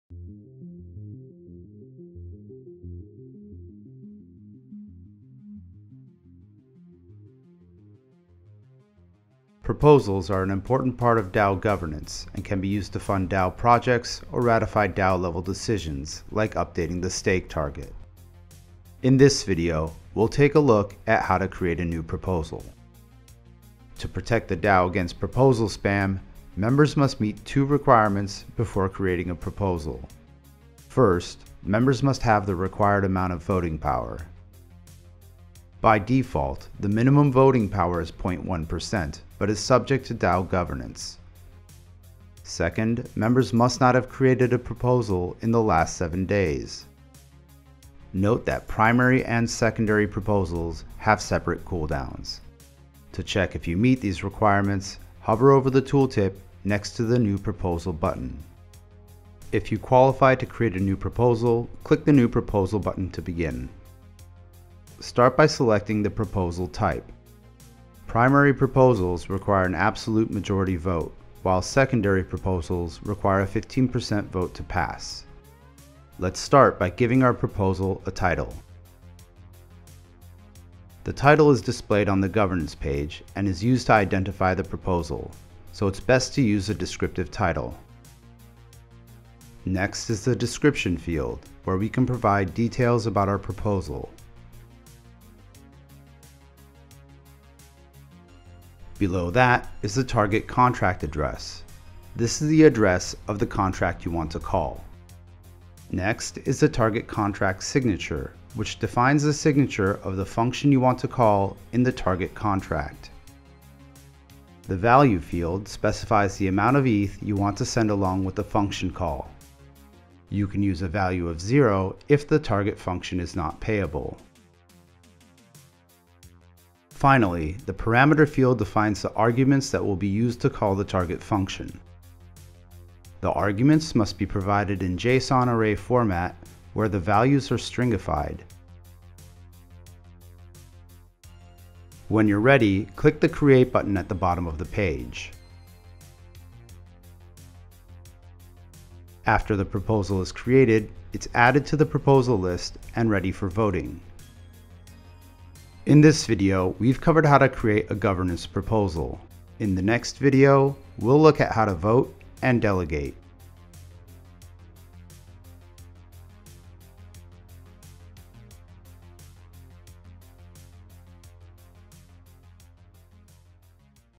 Authoratative DAO Tutorial - Create a Proposal
In this tutorial series the functionalities of the authoratative DAO will be explained. Our documentation can also be checked if anything remains unclear.